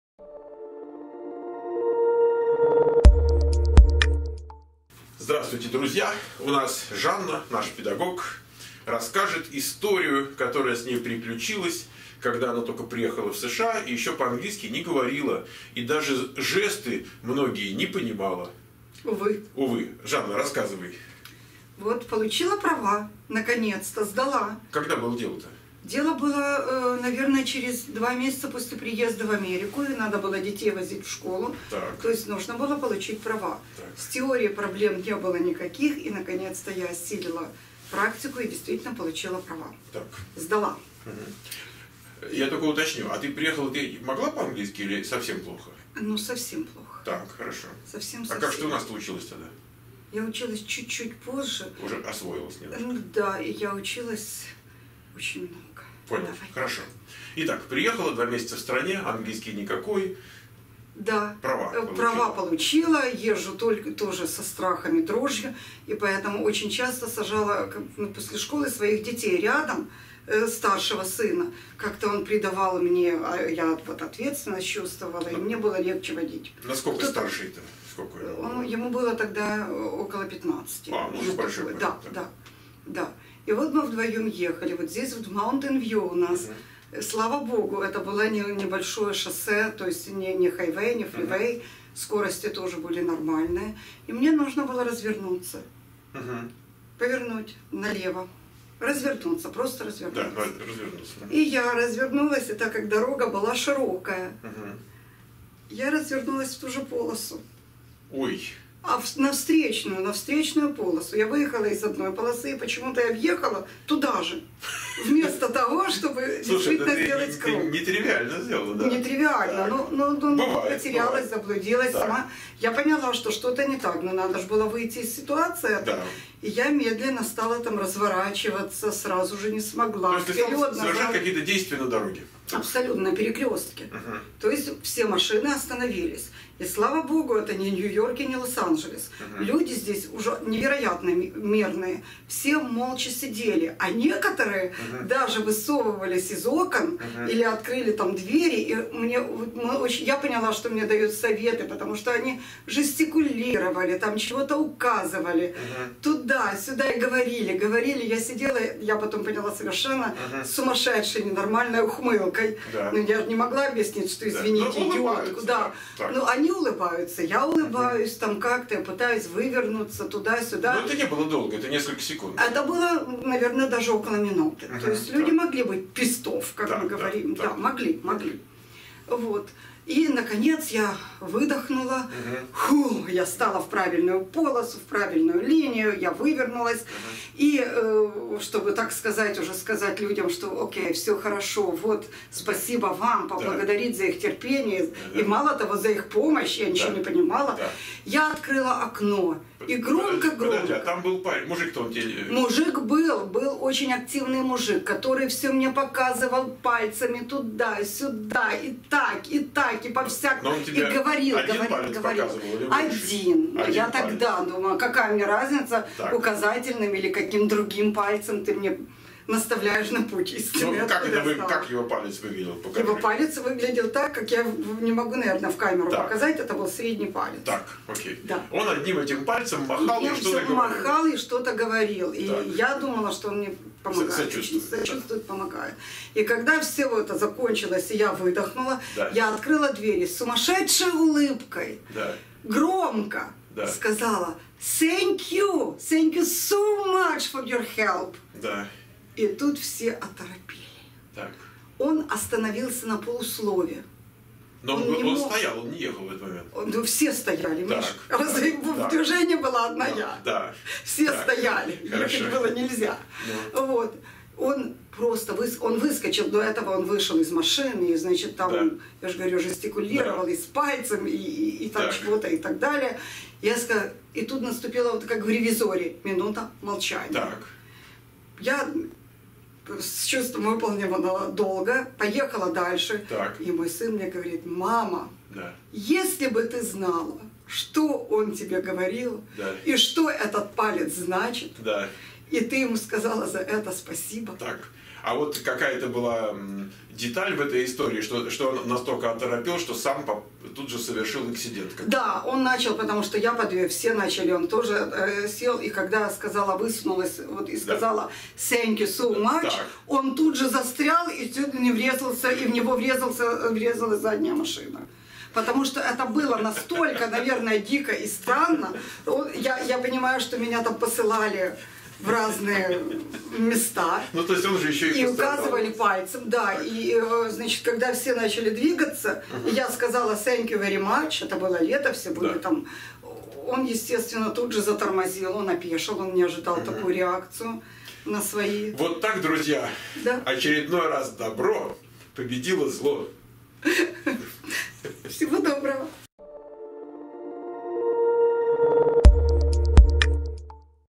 США 2803: Как Жанна на практике изучала язык жестов в Соединенных Штатах
Как Жанна на практике изучала язык жестов в Соединенных Штатах